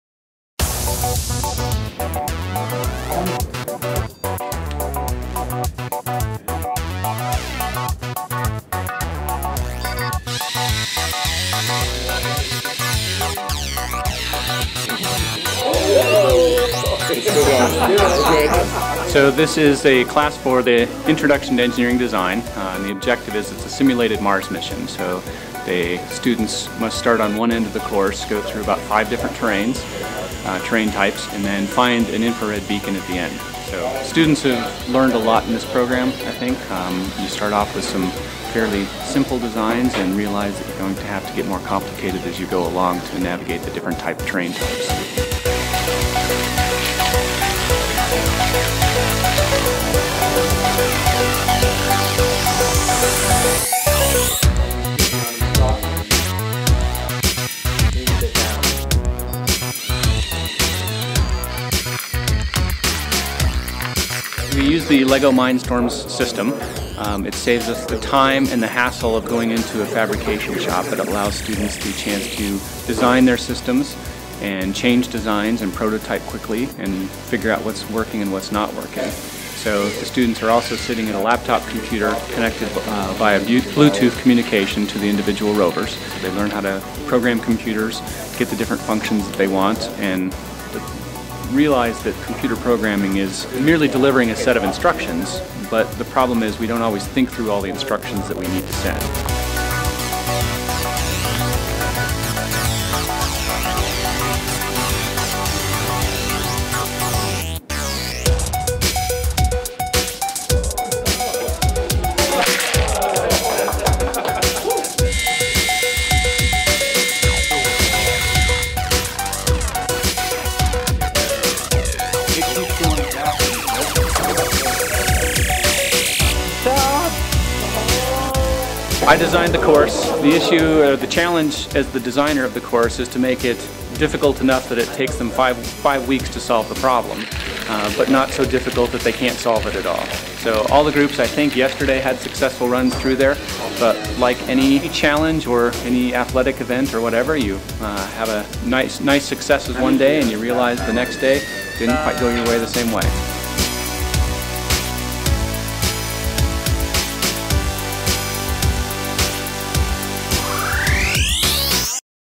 Intro to Engineering Design using Lego Mindstorm Robotics - Adams State University
Introduction to Engineering Design - simulated mars mission. These students put their creations to the test, tackling a variety of challenging terrain to track an infrared beacon. May the best robot win!